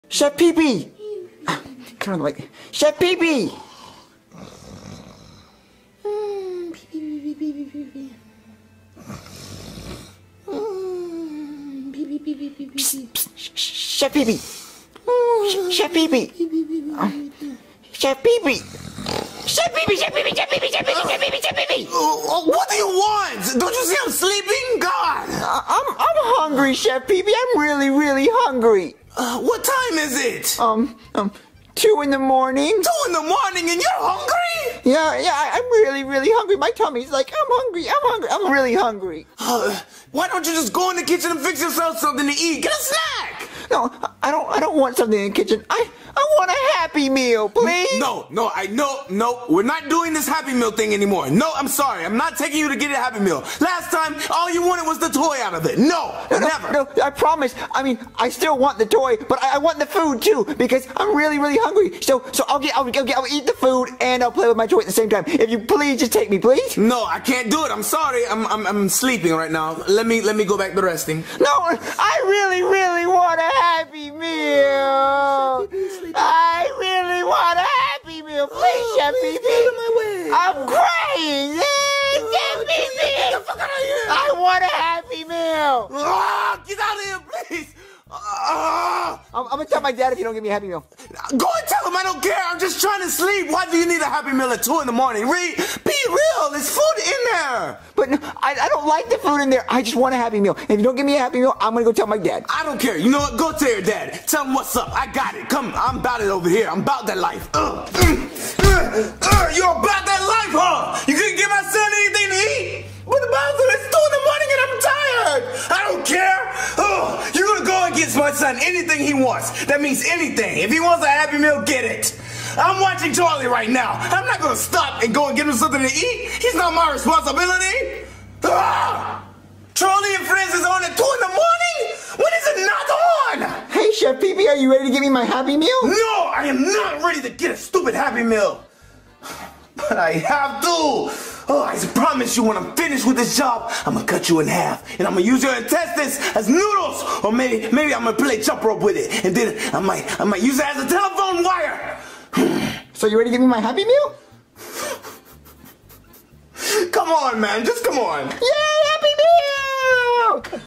Bowser Jr Waking Up Chef Pee Pee
Bowser Jr Wakes up Chef pee pee for a Happy Meal